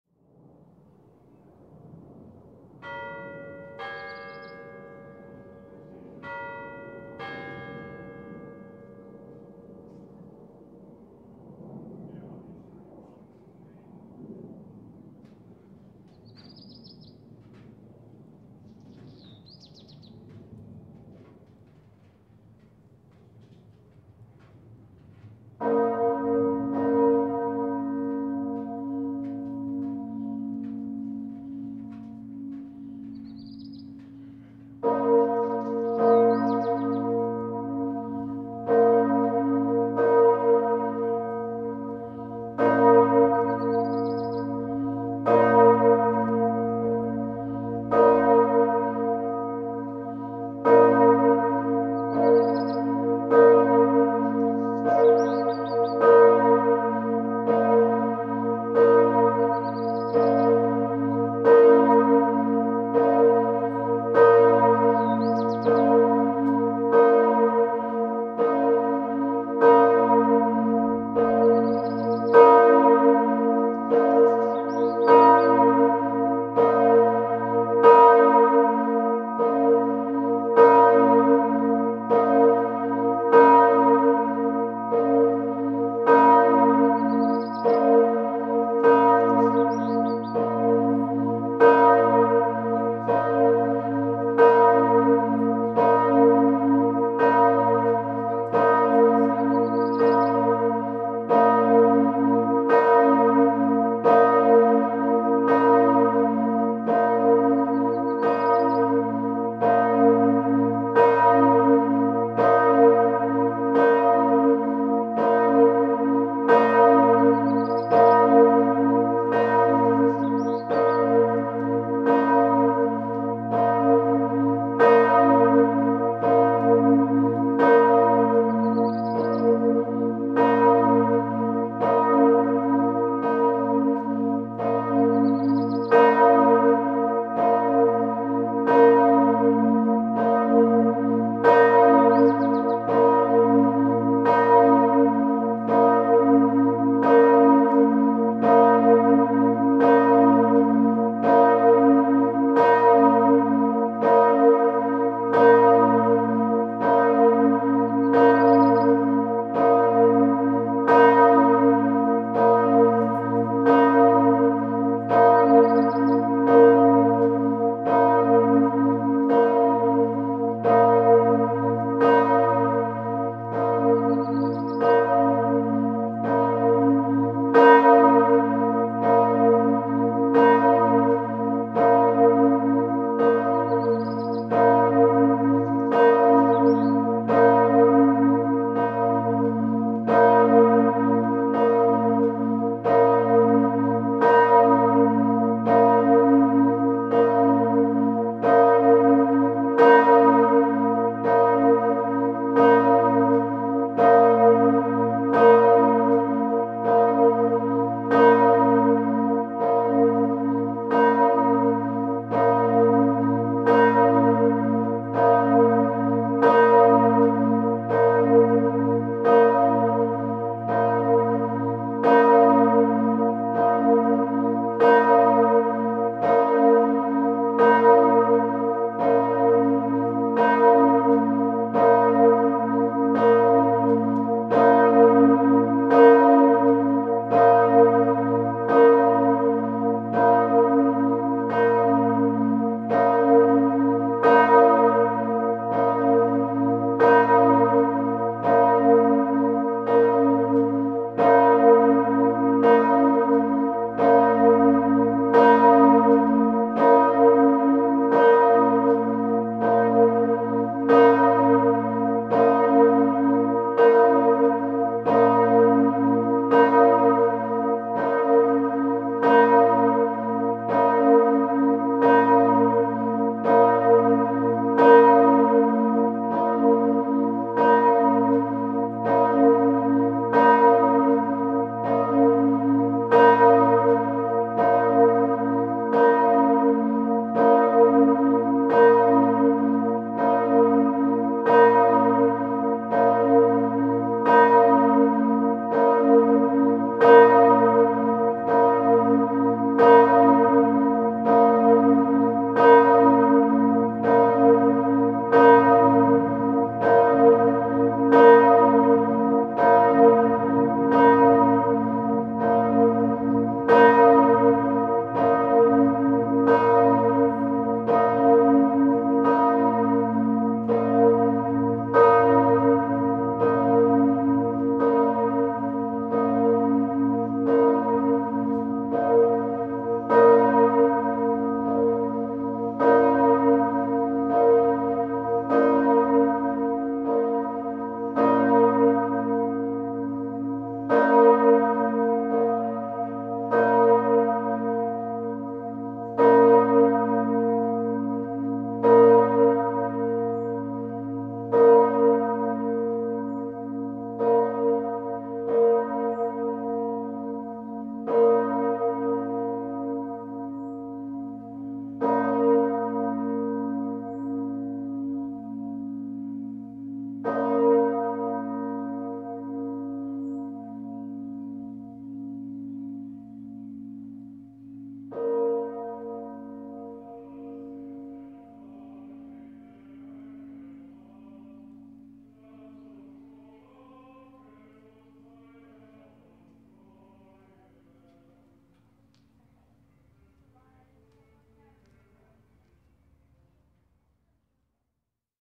Bad Homburg (HG) - St. Marien - Glocke 1
Vorläuten von St. Marien, Bad Homburg, zu einer feierlichen Ostervesper am Abend des Ostersonntags mit der Glocke 1.

Disposition:
1 b°, 3.717 kg - Maria vom Heiligsten Herzen Jesu/Barbara
2 d', 1.725 kg - Maria coronata/Julia
3 f', 965 kg - Maria assumpta/Theresia
4 g', 663 kg -- Maria dolorosa/Anna
5 a', 502 kg - Maria annutiata/Leonora
6 b', 412 kg - Maria immaculata/Margaretha
Die Glocken wurden 1894 von Petit & Gebr. Edelbrock, Gescher, gegossen, wobei die g'-Glocke 1949 neu gegossen werden musste.

Man beachte das Knarren des hölzernen Glockenstuhls am Anfang!

Video: M